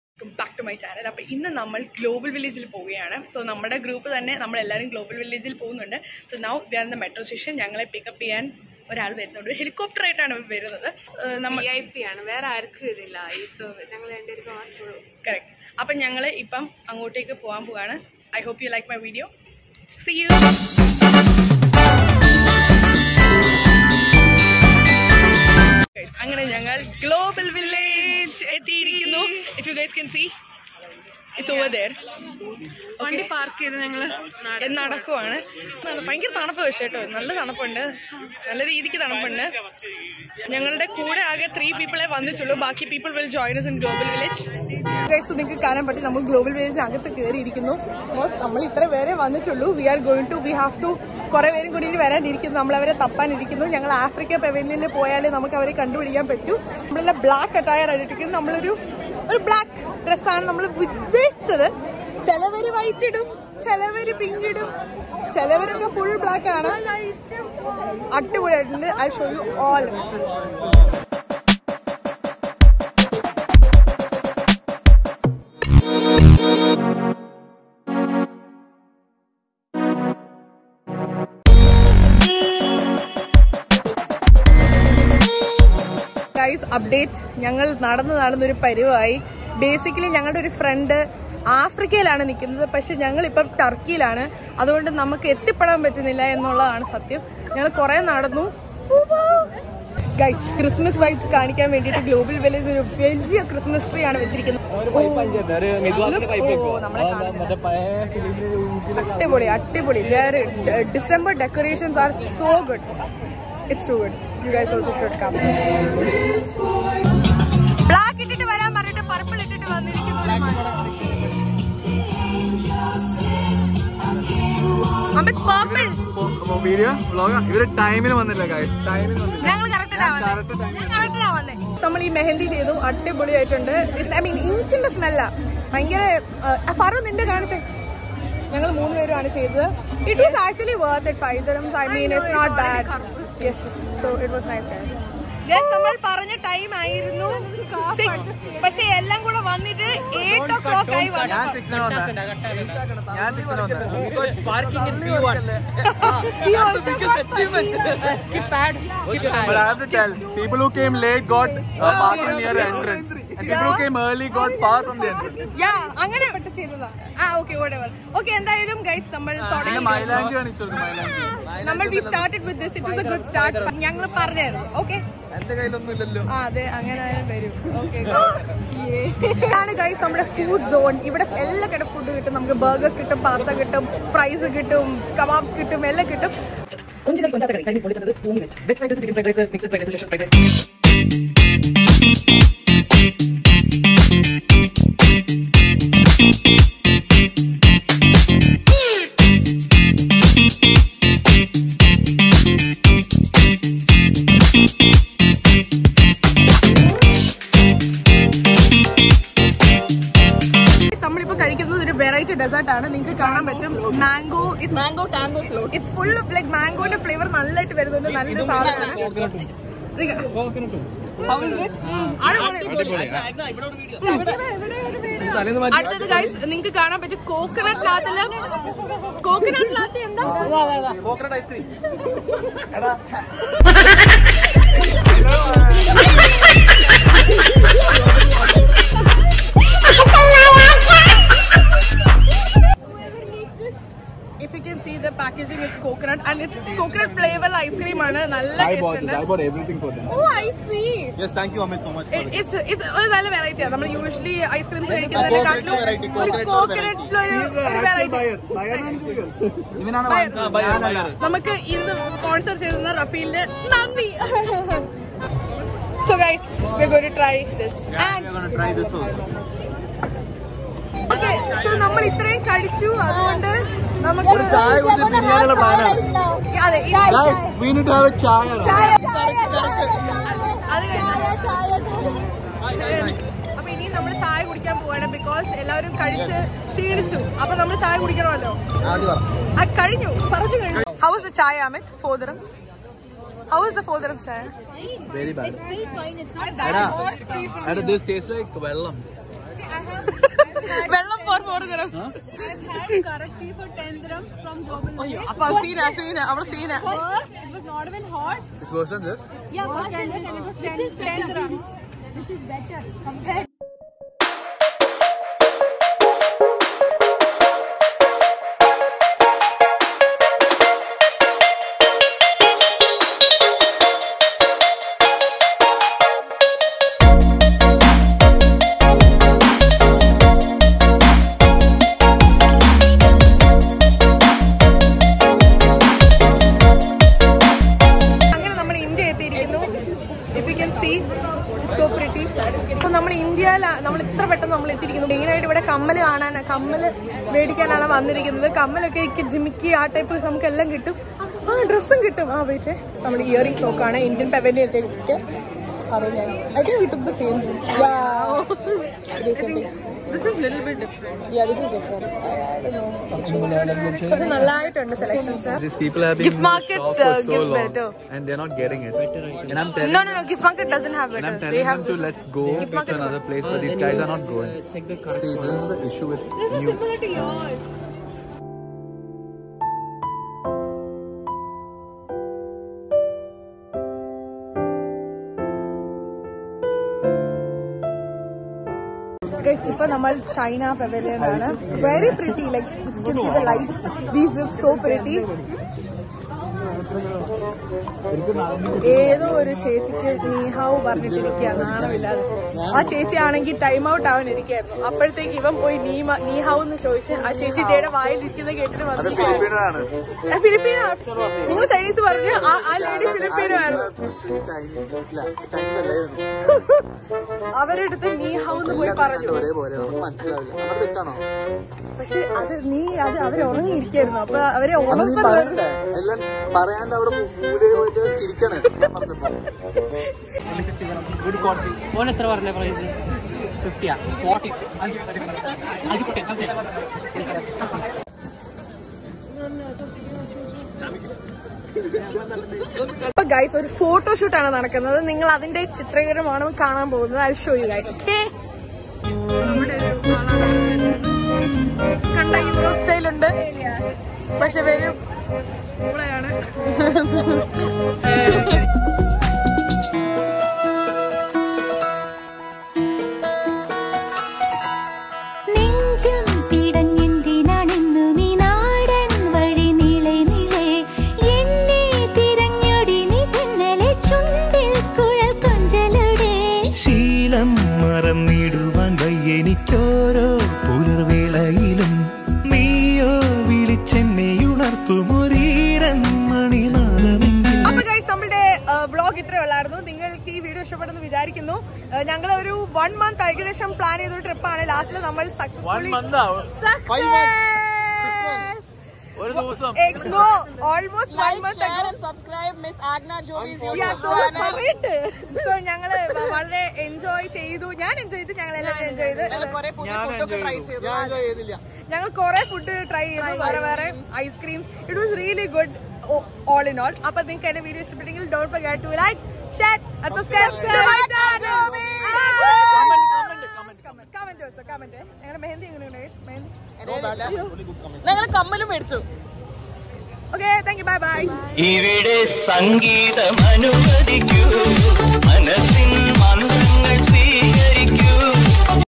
A Day at Global Village,Dubai🥐🧈🍗 || Ft. college mates👫🏃‍♀️🕺
Hellooo All!

Welcome back to yet another vlog!! We went to the global village and it was a great experience. Through this vlog I will show you all the places we visited the things we ate at global village!